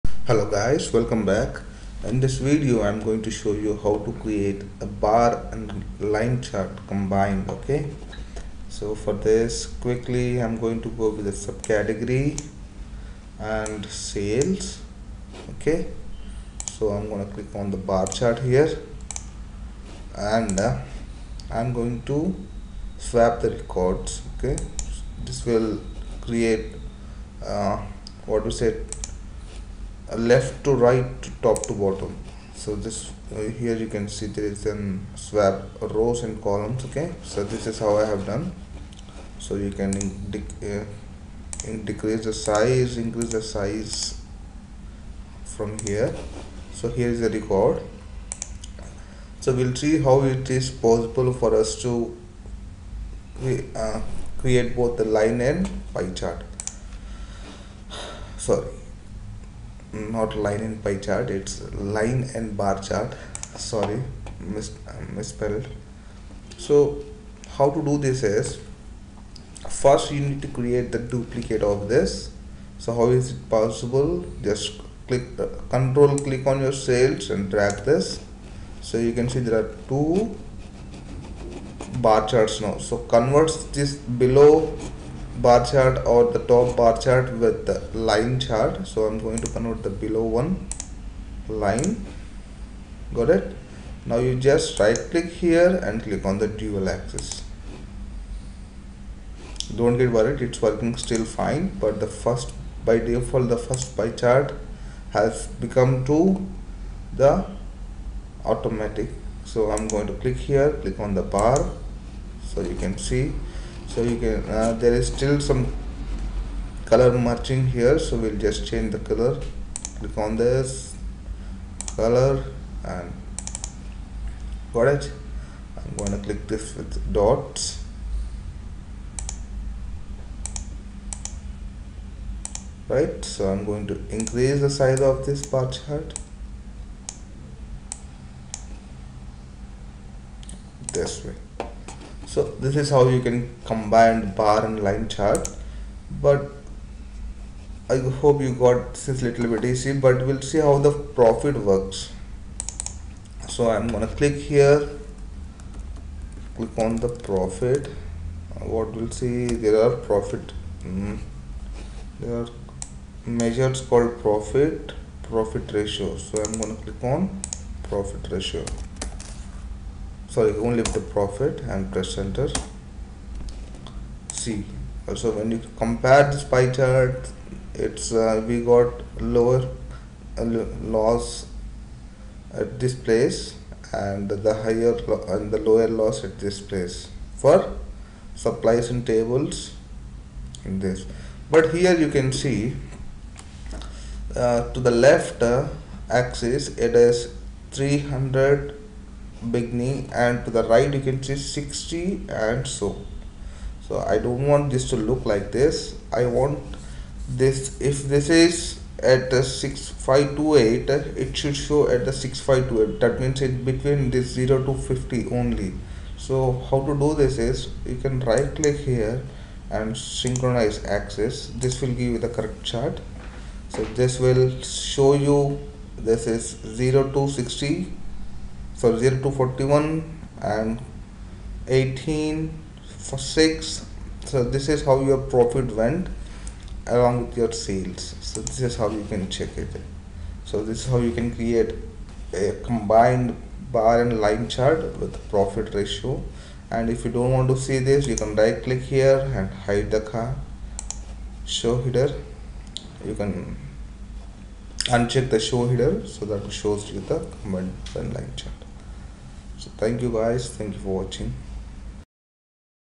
Bar and Line chart With Dual Axis, Synchronize Axis in Tableau Desktop - Intact Abode
How to Create Bar and Line chart With Dual Axis Synchronize Axis  in Tableau Desktop